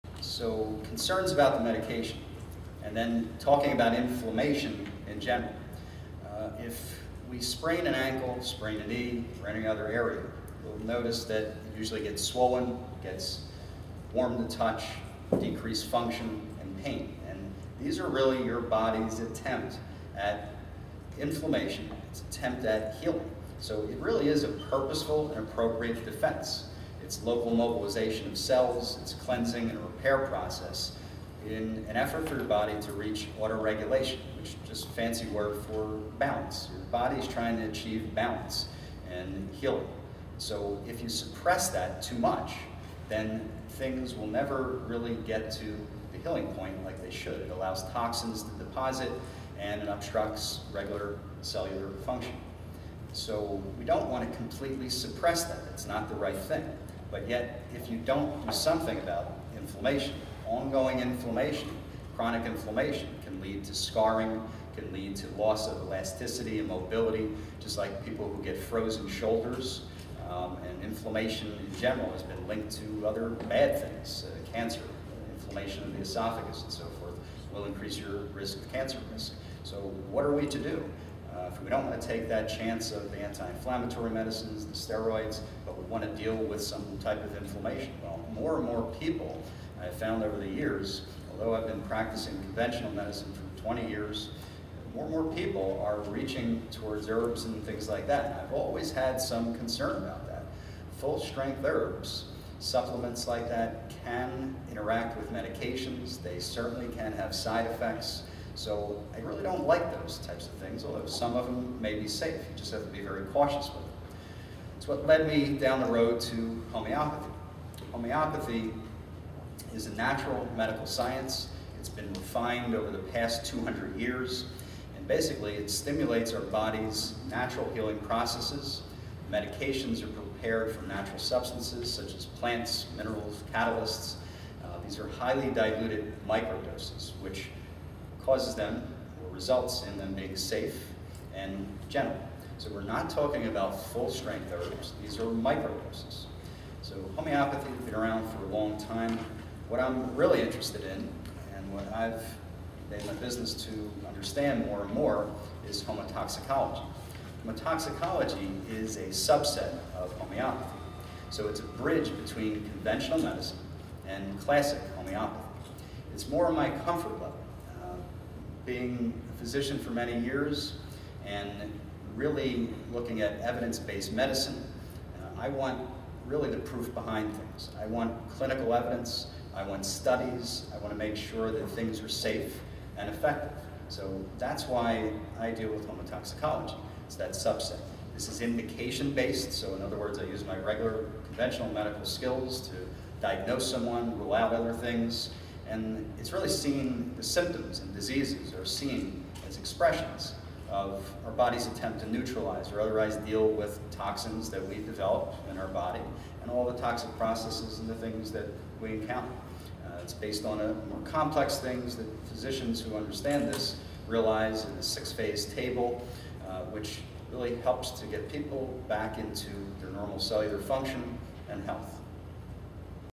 Dr. Robert Davis lecturing about Inflammation
Dr. Robert Davis, a board certified family physician, co-founder of Integrated Family Medicine in Voorhees, NJ, lecturing at Virtua Health System about Natural Treatment for Inflammation. October 2011